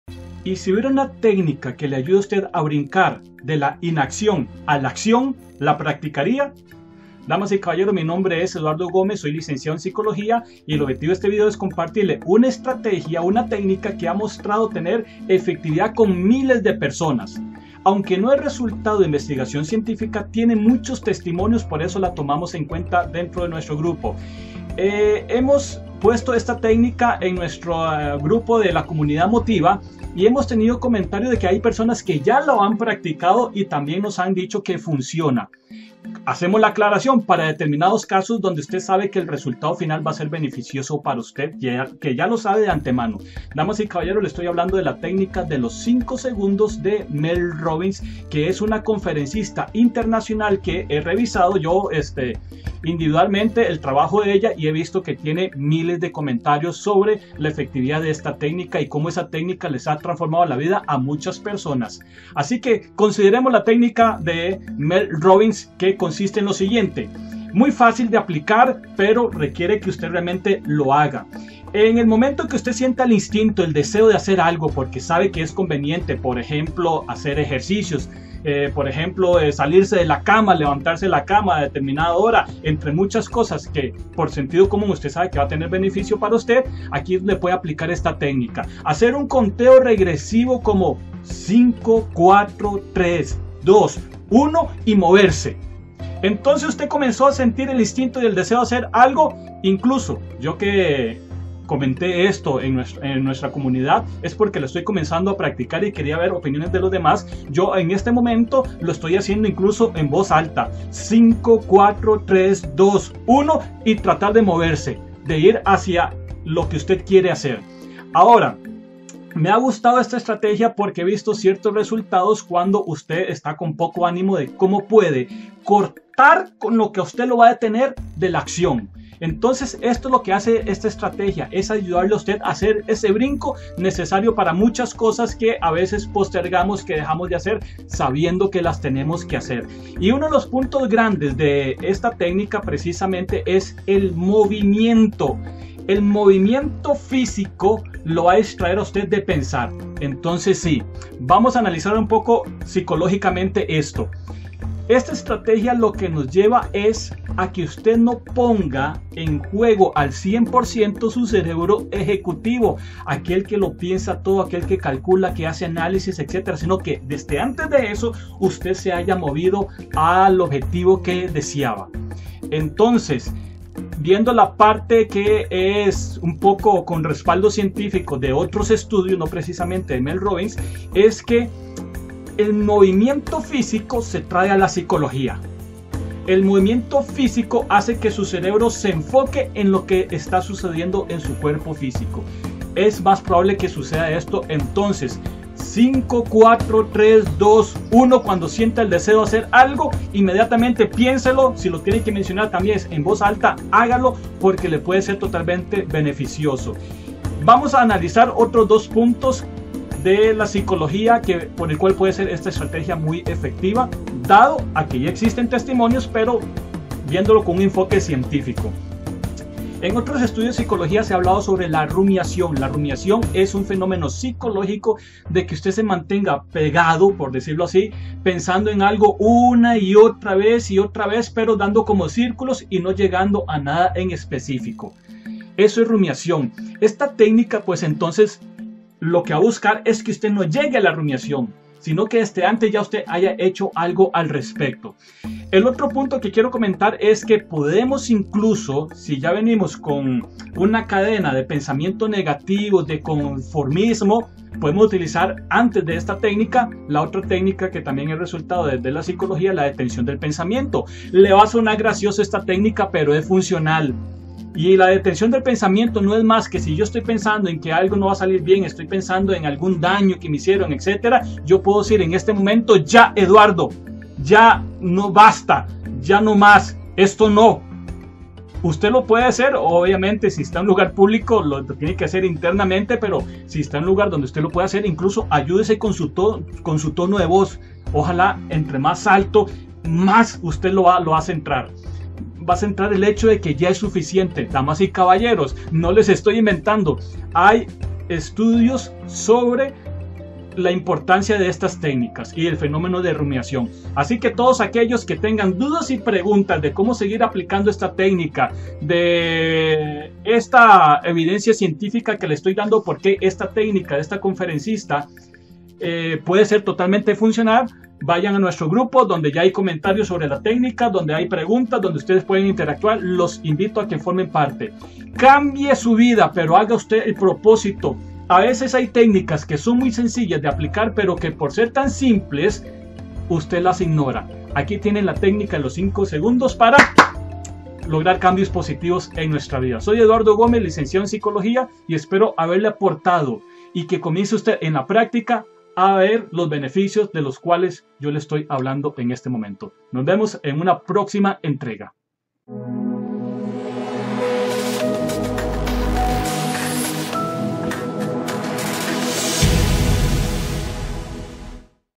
La técnica motivacional 5 segundos de Mel Robbins
El poder de los 5 segundos de la conferencista Mel Robbins y su análisis psicológico de su funcionalidad sé valiente en el día a día y transforma tu vida.